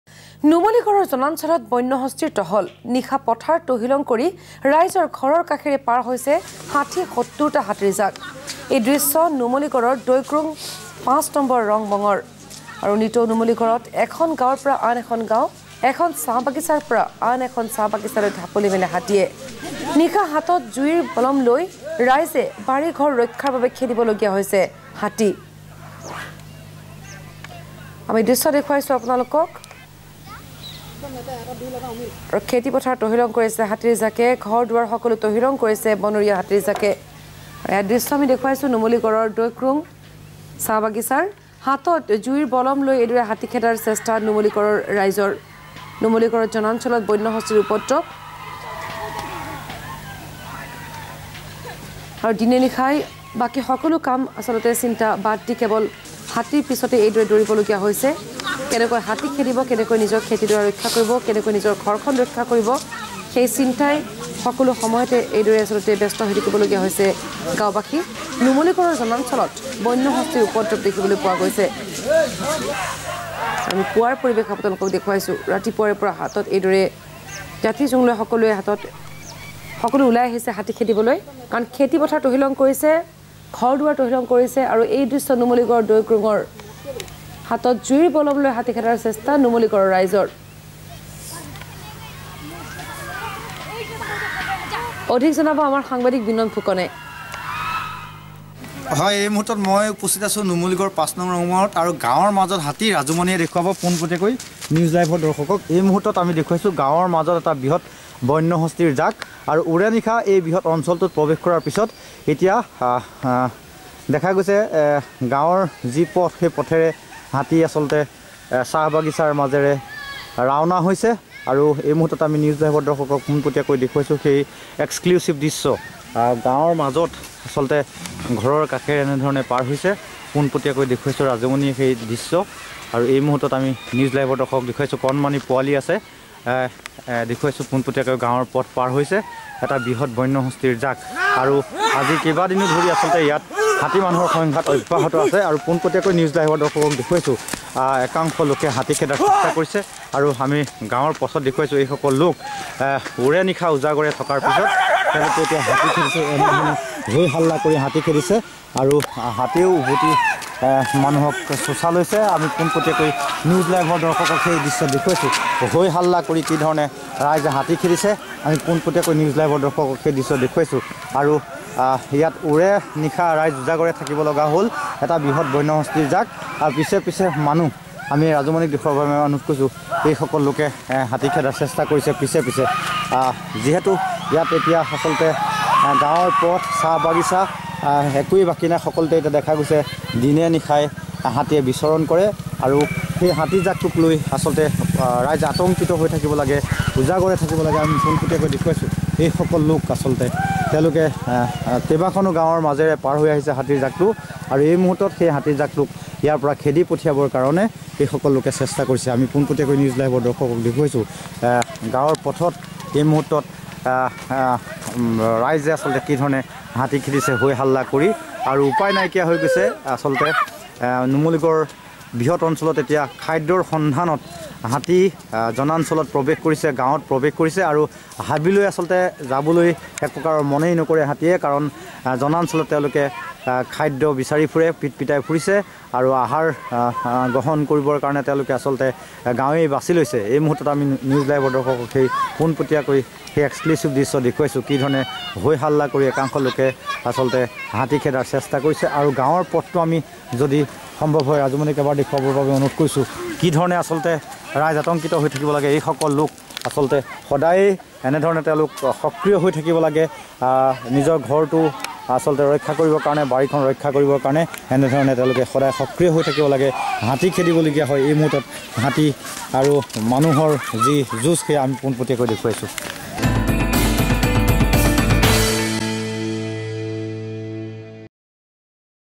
নুমলীগড়ত বন্যহস্তীৰ উপদ্ৰৱ। নিশা হাতত জুইৰ বলম লৈ ৰাইজে বাৰী, ঘৰ ৰক্ষাৰ বাবে খেদিব লগা হৈছে হাতী।
নুমলীগড়ৰ জনাঞ্চলত বন্যহস্তীৰ উপদ্ৰৱ। খেতিপথাৰ তহিলং কৰিছে ৬০-৭০ জনীয়া হাতীৰ জাক। নিশা হাতত জুইৰ বলম লৈ ৰাইজে বাৰী, ঘৰ ৰক্ষাৰ বাবে খেদিব লগা হৈছে হাতী।